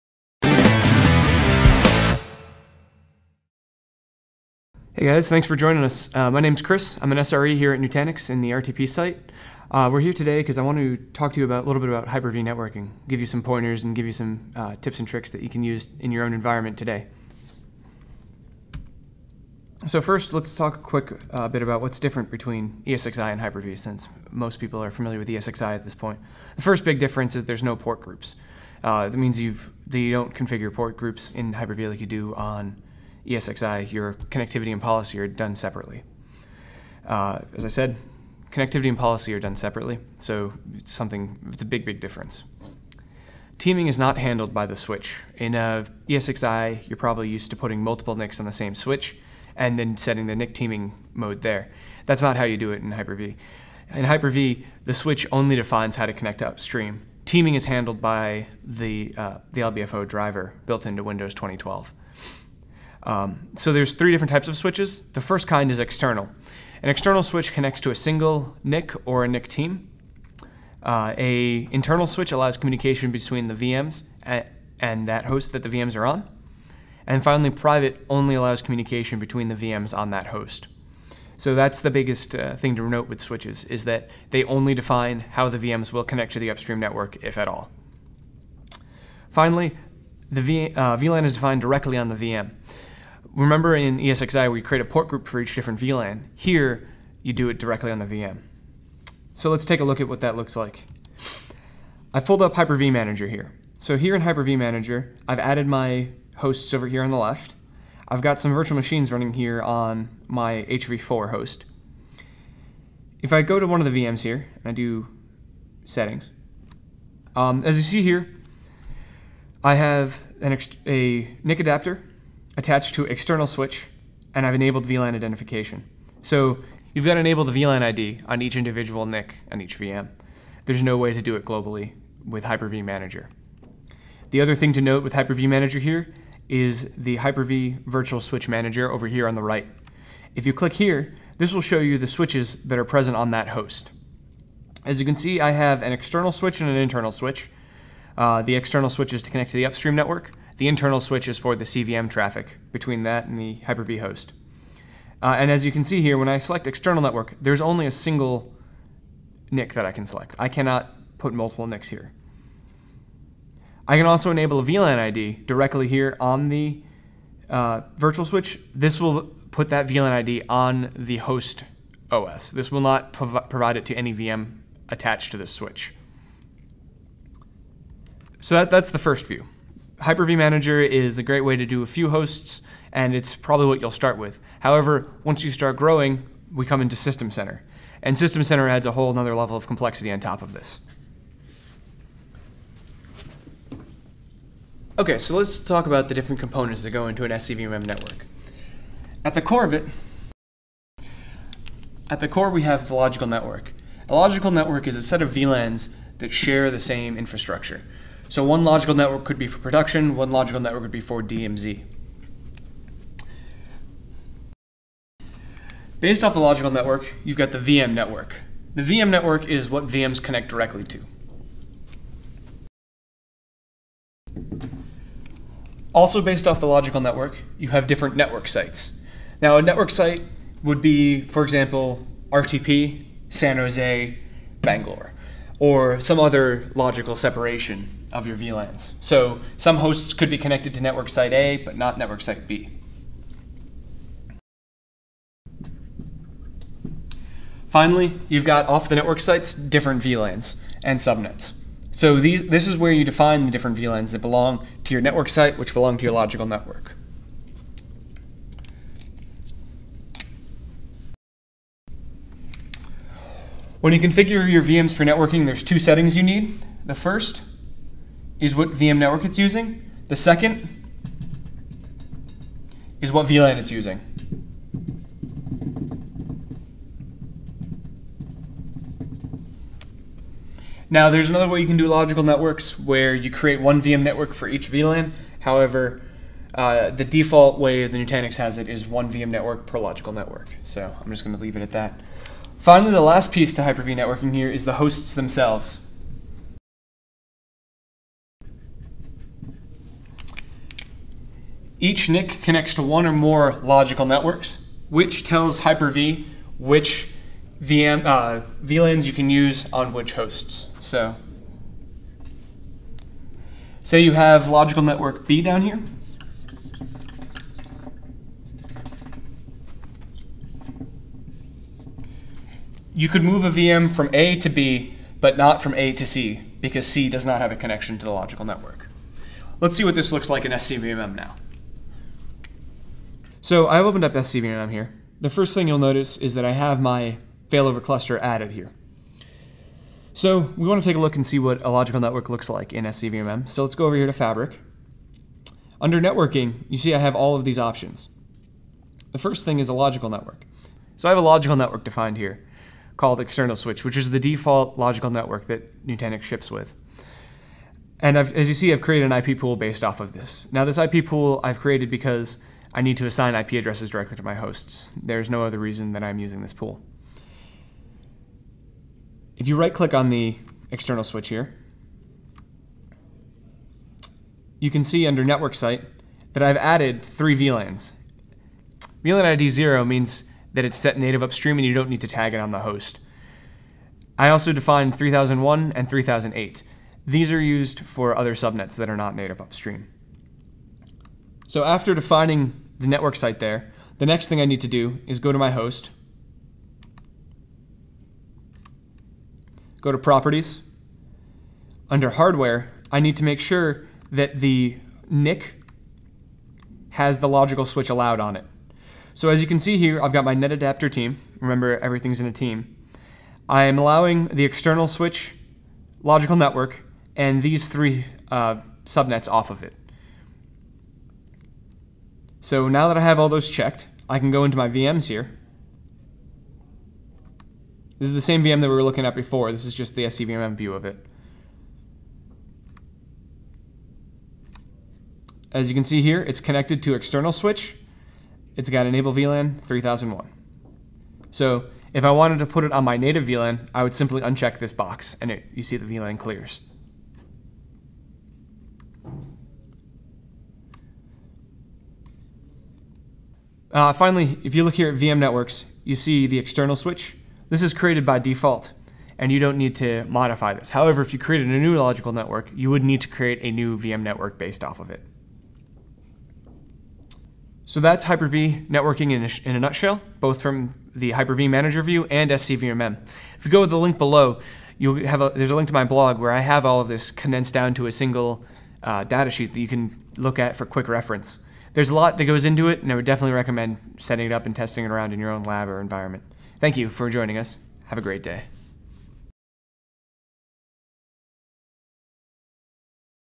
Tech TopX: Hyper-V Networking
Chris Brown describes Hyper-V networking tips and tricks including the differences between ESXi and Hyper-V connectivity, policy, and teaming.